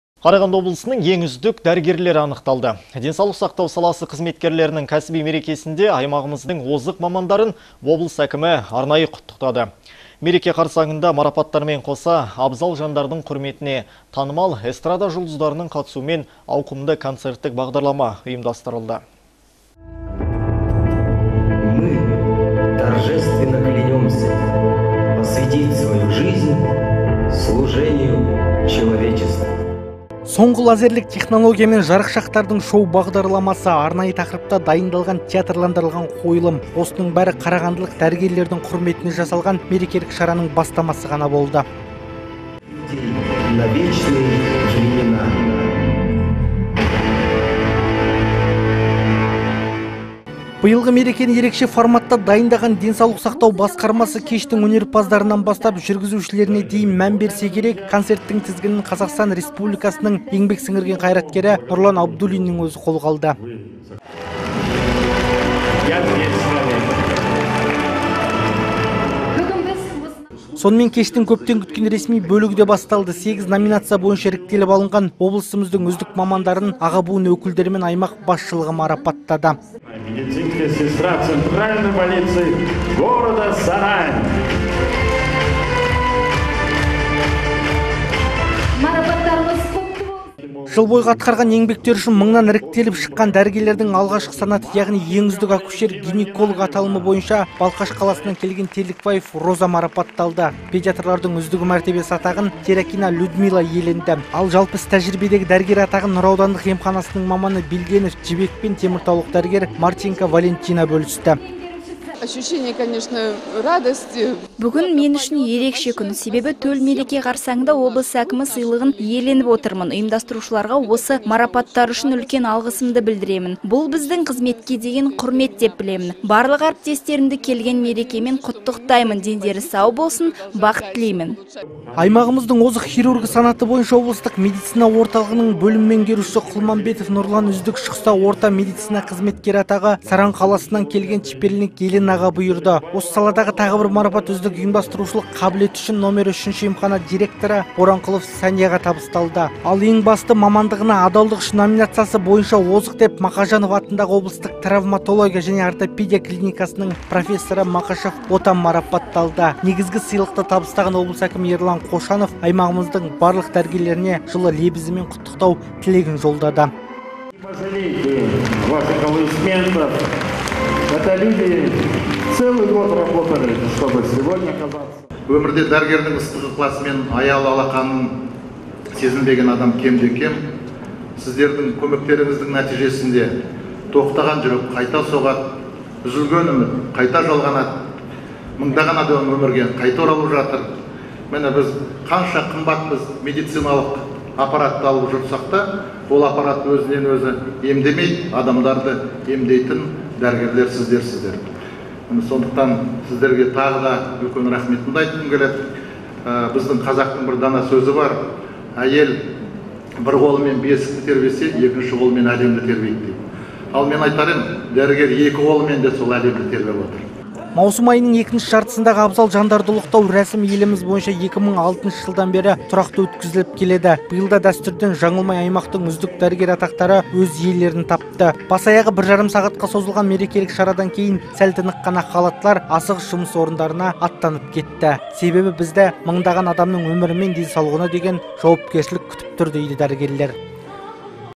Қарағанды облысының ең үздік дәрігерлері анықталды.
Денсаулық сақтау саласы қызметкерлерінің кәсіби мерекесінде аймағымыздың озық  мамандарын  облыс әкімі  арнайы құттықтады. Мереке қарсаңында  марапаттар мен қоса абзал жандардың  құрметіне танымал  эстарда жұлдыздарының қатысумен  ауқымды концерттік бағдарламада ұсынылды.
Соңғы лазерлік технология мен жарышақтардың  шоу бағдарламасы. Арнайы тақырыпта дайындалған театрландырылған қойылым. Осының бәрі қарағандылық дәрігерлердің құрметіне жасалған  мерекелік шараның бастамасы ғана болатын.
Биылғы мерекені ерекше форматта дайындаған денсаулық сақтау басқарамасы  кештің өнерпаздарынан бастап жүргізушілеріне дейін  мән берсе керек концерт тізгінін ҚР  еңбек сіңірген қайраткері Нұрлан Абдулиннің өзі қолға алды.
Сонымен кештің көптен күткен ресми бөлігіде басталды. 8 номинация бойынша іріктеліп алынған облысымыздың үздік мамандарын аға буын өкілдері мен  аймақ басшылығы марапаттады.
Жыл бойғы атқарған еңбектері үшін мыңнан іріктеліп шыққан дәрігерлердің алғашқы санаты яғни ең үздік окушер-гинеколог атағы Балхаш қаласынан келген Терлікбаева Розаға бұйырды. Педиатрлардың үздігі мәртебесі атағын  Терекина  Людмила иеленді. Ал жалпы іс тәжірбиедегі  дәрігер атағын  Нұра аудандық  емханасының  маманы  Билденов Жібек пен Теміртаулық дәрігер Марченко Валентина бөлісті.
Аймағымыздың озық хирургы санаты бойынша облыстық медицина орталығының бөлім меңгерушісі  Құлмағанбетов Нұрлан  үздік шықса, орта медицина қызметкері атағы Саран қаласынан келген  Чперлинк Еленаға бұйырды. Осы саладағы тағы бір марапат үздік ұйымдастырушылық қабілеті үшін №3 емхана дириторы  Боранқұлова Сәнияға табысталды. Ал ең басты  «мамандығына адалдығы үшін» номинациясы бойынша озық деп Мақажанов атындағы облыстық травмотология және ортопедия клиникасының профессоры Мақышев Отан марапатталды.Негізгі сыйлықты табыстаған облыс әкімі Ерлан Қошанов аймағымыздың барлық дәрігерлерінеде жылы лебізі мен  құттықтау тілегін жолдады.
Маусым айының екінші жартысындағы абзал жандарды ұлықтау рәсімі еліміз бойынша 2006 жылдан бері тұрақты өткізіліп келеді. Биыл да дәстүрден жаңылмай  аймақтың  үздік  дәрігер атақтары өз иелерін  тапты. Бас аяғы бір жарым сағатқа созылған мерекелік шарадан кейін сәл тыныққан  ақ халаттылар  асығыс жұмыс орындарына аттанды. Себебі «бізді  мыңдаған адамның өмірі мен  денсаулығына деген жауапкершілік күтіп тұр,- дейді  дәрігерлер.